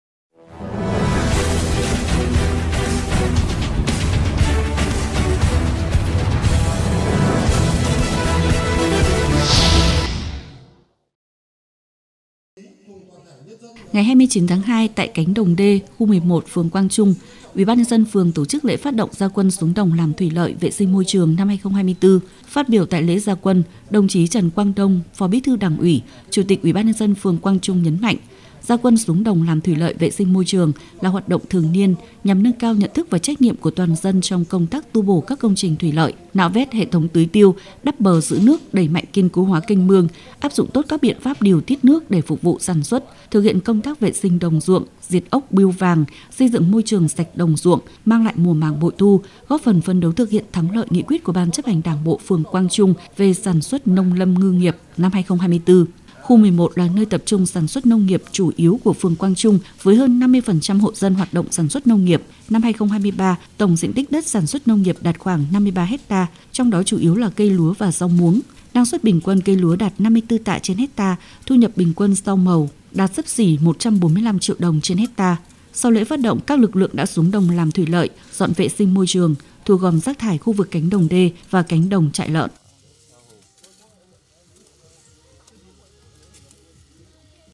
Phường Quang Trung phát động ra quân làm thủy lợi, vệ sinh môi trường năm 2024
Ngày 29/2, tại cánh đồng D, khu 11, phường Quang Trung, UBND phường tổ chức Lễ phát động ra quân xuống đồng làm thủy lợi, vệ sinh môi trường năm 2024. Dự lễ phát động có lãnh đạo Hội Nông dân thành phố, lãnh đạo Đảng ủy, HĐND, UBND, Uỷ ban MTTQ và các đoàn thể phường cùng đông đảo nhân dân trên địa bàn khu dân cư.